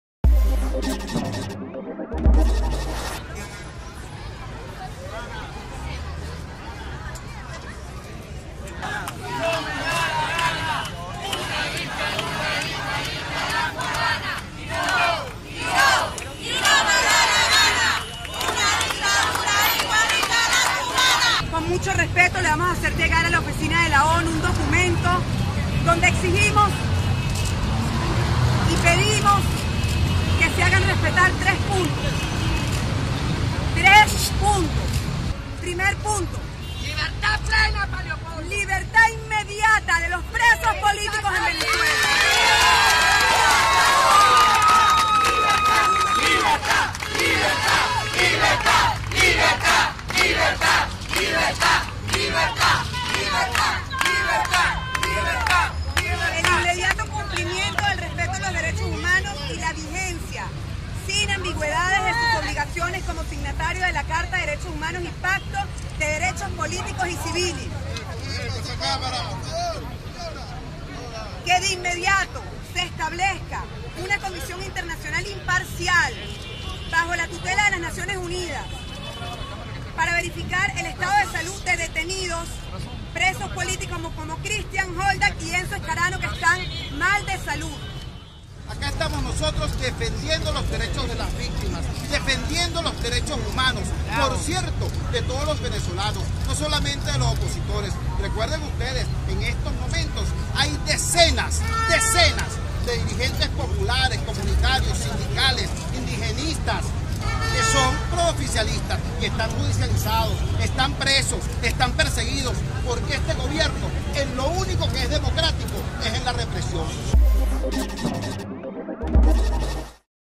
Lilian Tintori fue a la ONU acompañada por "Chuo" Torrealba
Entregaron un documento con tres exigencias puntuales, para exigir la libertad de los presos políticos